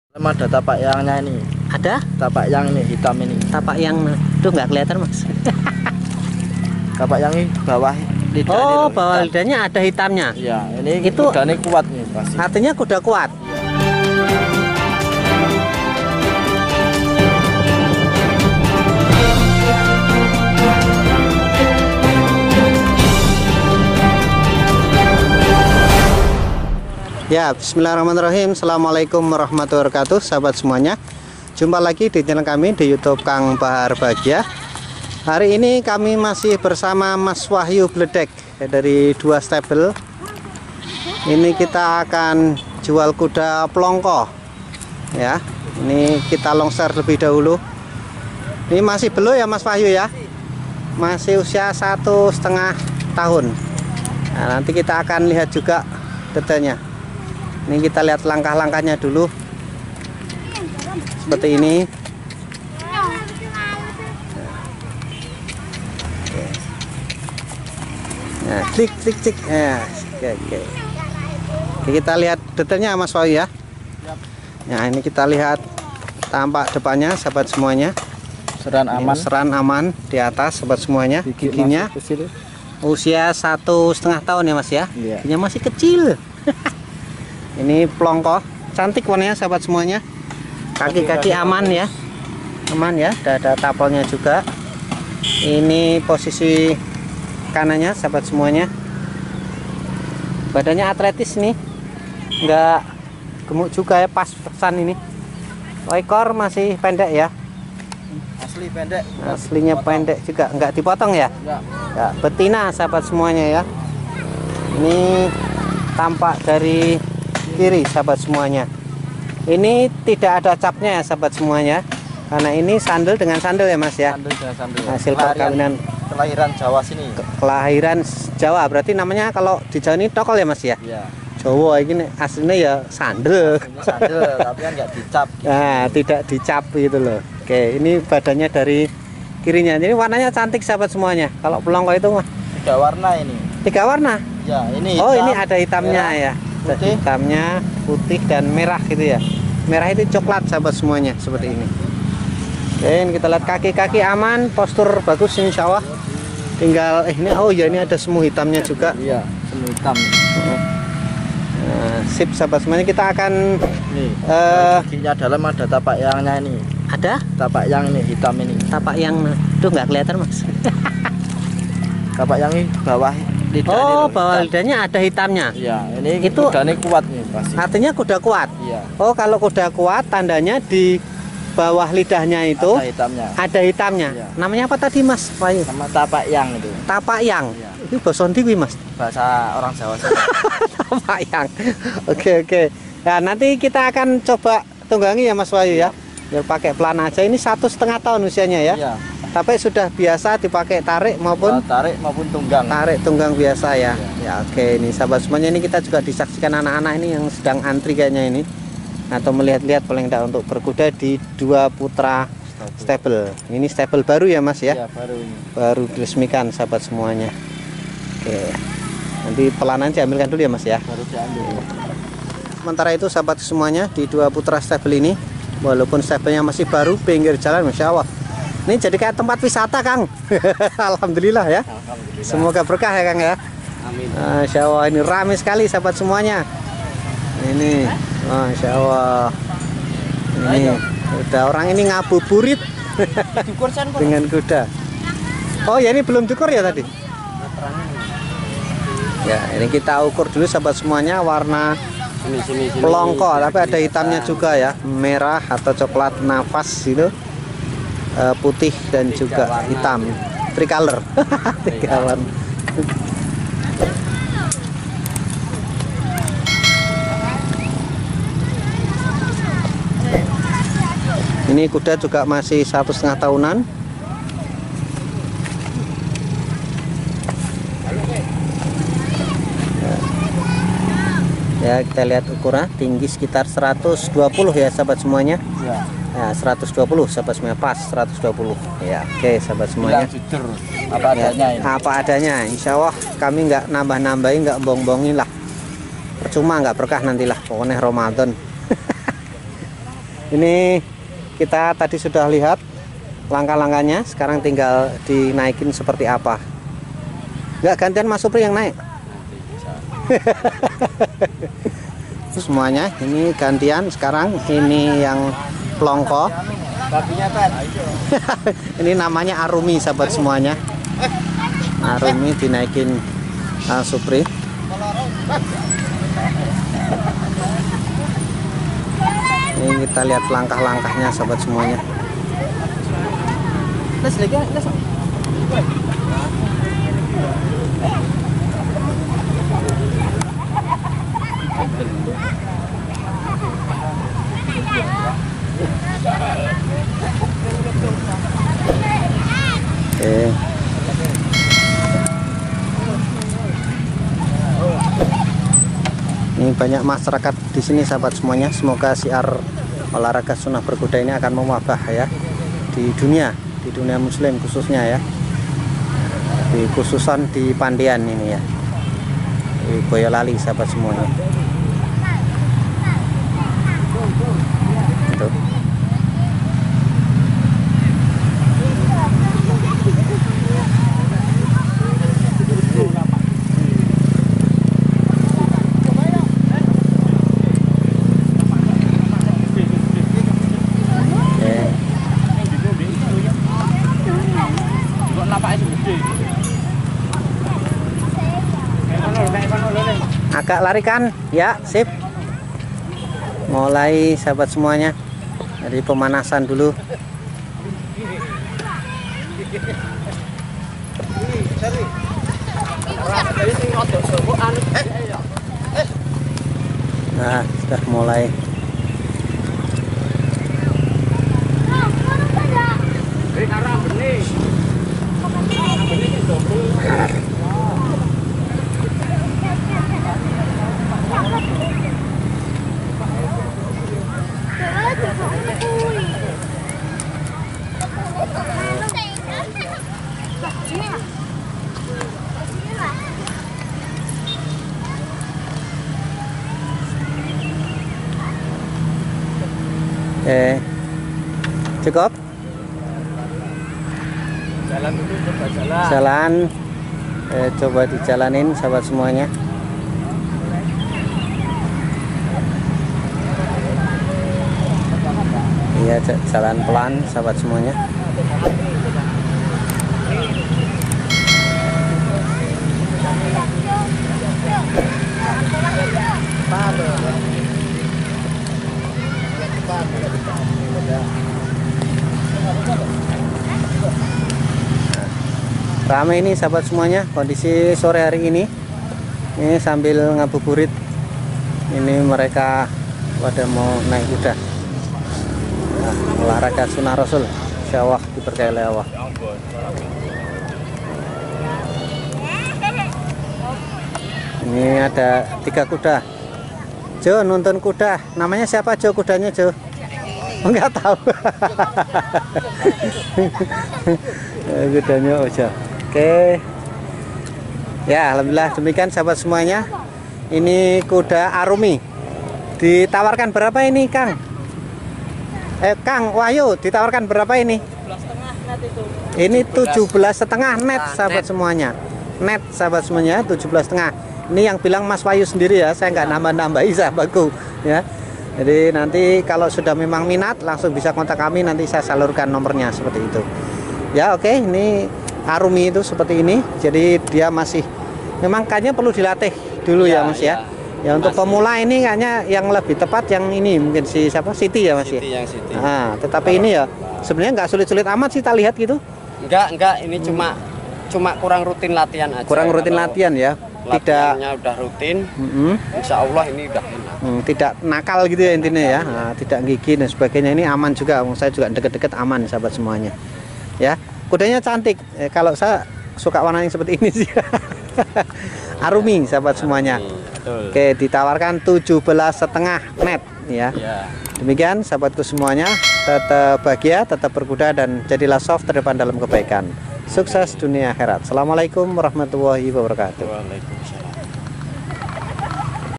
Jual Kuda Plongko Betina Arumi. Ada tapakyang nya juga lho...
Kali ini kami jual kuda plongko betina Arumi. Kuda ini usia masih1,5 tahun dengan tinggi sementara 120 cm.  Di mulutnya, tepatnya di bawah lidahnya ada tapakyang nya alias warna hitam di mulutnya. Tapakyang ini berdasarkan pengalaman merupakan kuda yang kuat fisiknya, Untuk lebih jelasnya, saksikan hingga tuntas ya.
Permintaan liputan, endorse, review kuda dan panahan silakan wa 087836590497
Tetap Bahagia
Tetap Berkuda
Dan Jadilah Shof Terdepan dalam Kebaikan ...SUKSES DUNIA AKHIRAT !!!